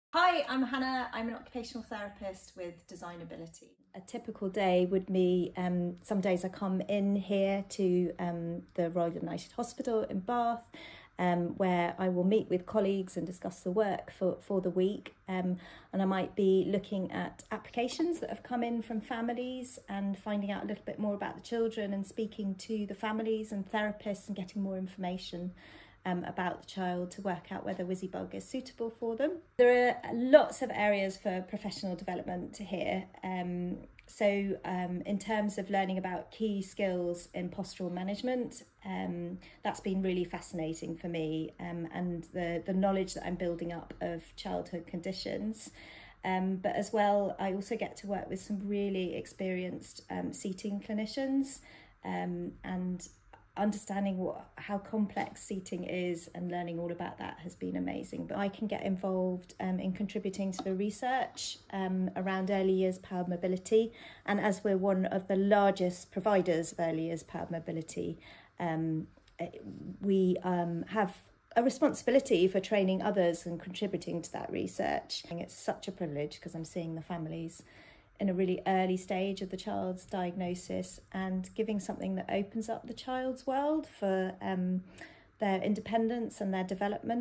Working as an Occupational Therapist for Designability
Being a Designability OT isn’t like other OT roles so we asked Hannah to share a very small insight into why working here in this kind of role is so unique and rewarding.

You’ll work as a vital member of the team across all departments of the organisation – whilst the Wizzybug Loan Scheme will be at the heart of your work, you’ll interact with our design and engineering, production and marketing and fundraising teams.

You will speak regularly with families about their application for a Wizzybug and will continue to support them with their child’s driving skills throughout their loan. But you will also get involved with attending exhibitions and conferences where you can share more about Designability and expand your professional development.

It’s true that no two days at Designability are ever the same!